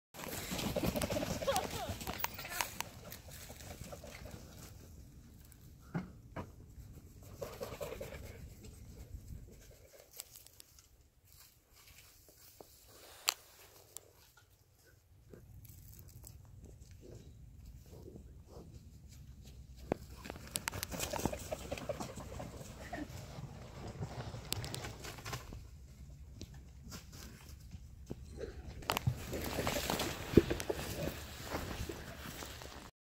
Tiger chases vultures away from alpaca lunch
Rajah Tiger didn't appreciate vultures trying to eat his lunch! Carolina Tiger Rescue accepts some livestock donations, and a local alpaca farmer brought in one that had died of natural causes. Rajah got the torso!